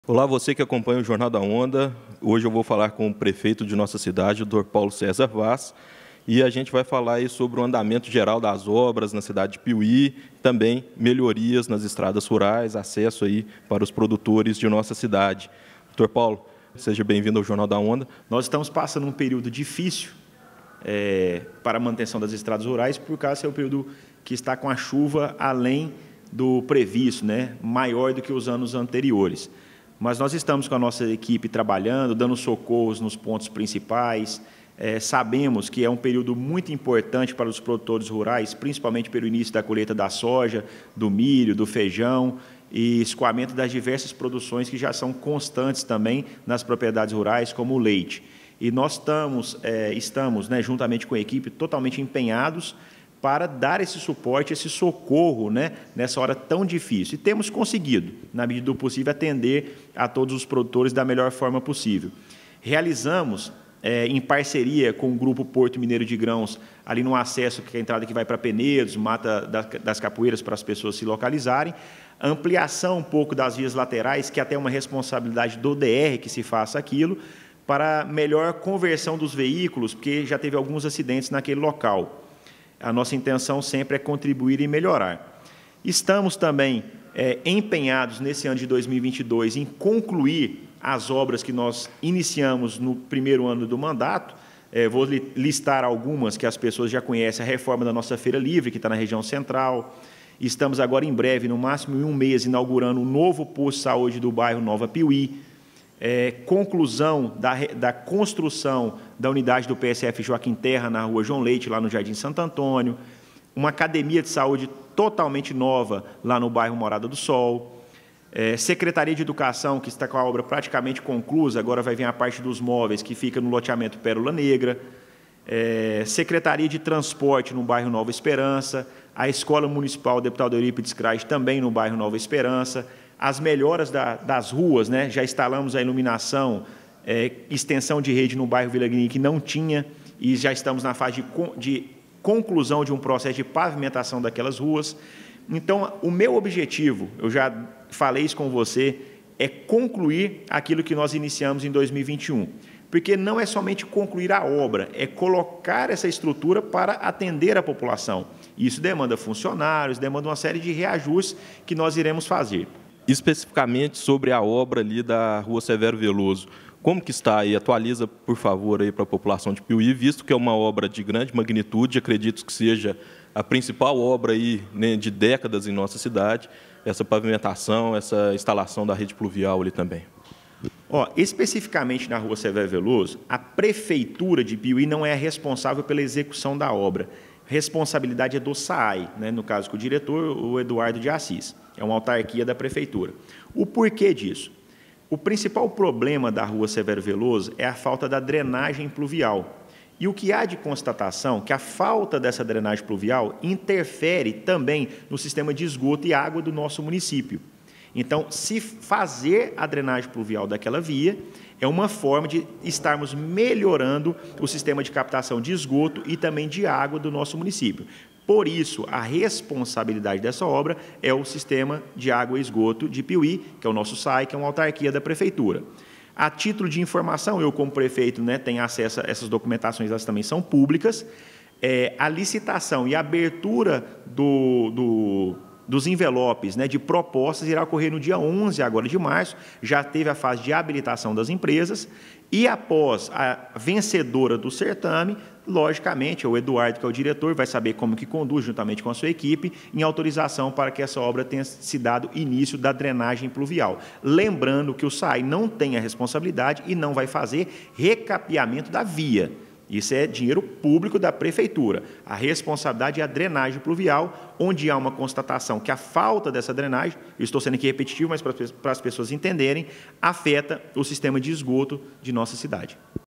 PREFEITO DE PIUMHI FALA DO ANDAMENTO DAS OBRAS NA CIDADE E MELHORIAS NAS ESTRADAS RURAIS
| PIUMHI |